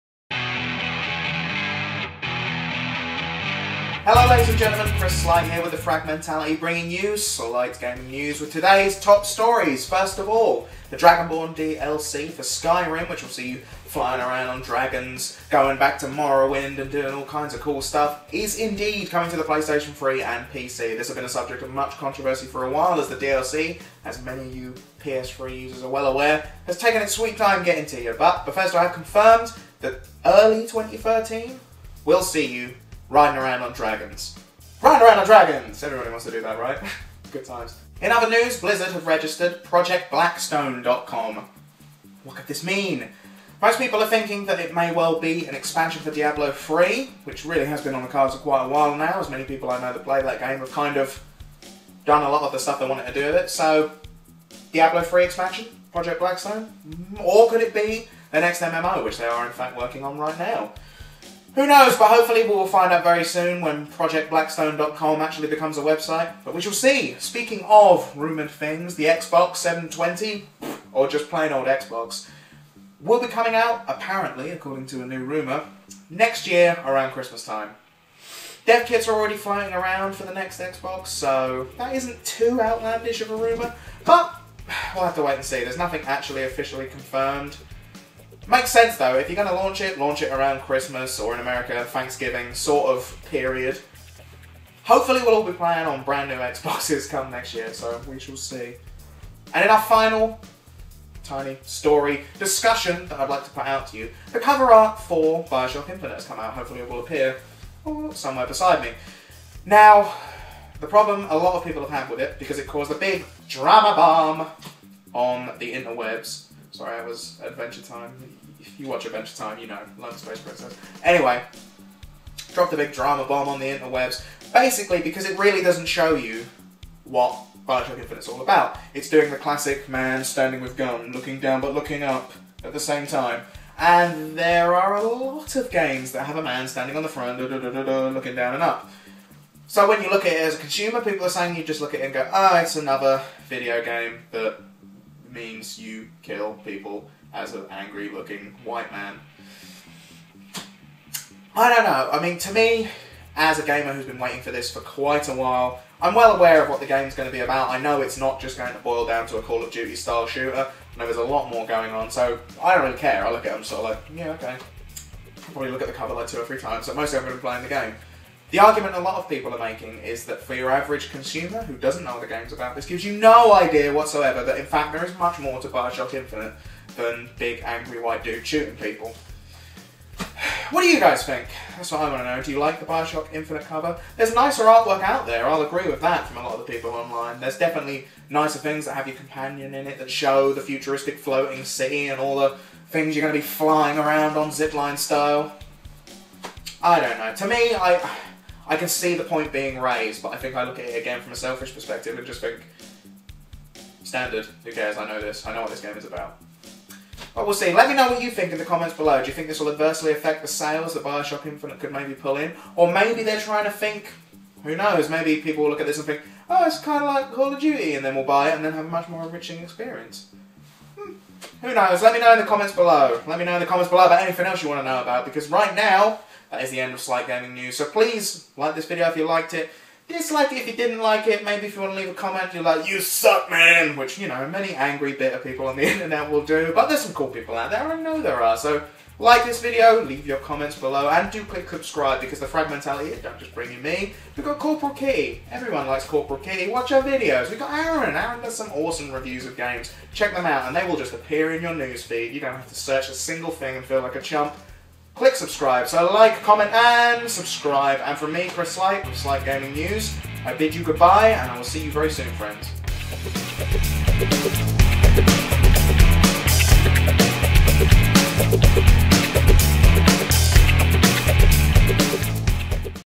XBOX 720 launch trailer LEAKED & Blizards Project Blackstone
Chris looks at the Dragonborn DLC PS3 & PC release dates, next Gen Xbox release rumours, Blizards Project Blackstone and the controversy behind Bioshock Infinite's cover.